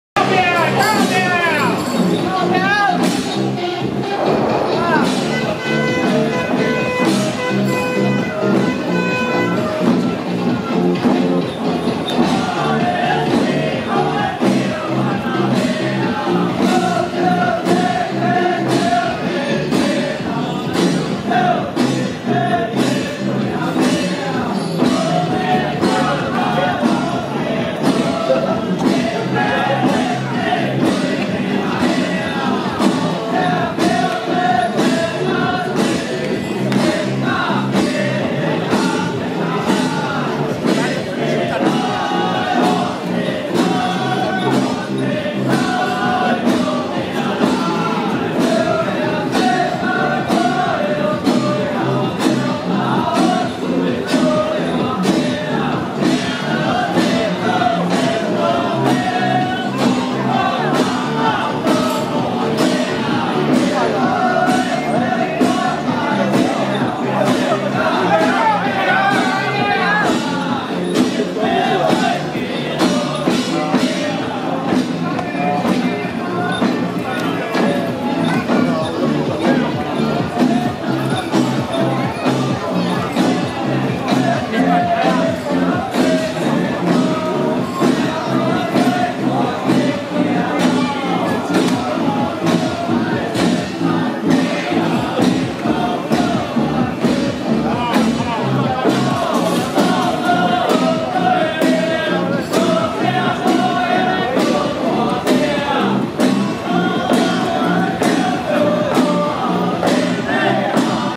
105.6.12台北勝靈宮進香回駕~【王爺接駕勝靈宮太子武轎】
2016/6/12台北勝靈宮南巡進香回駕遶境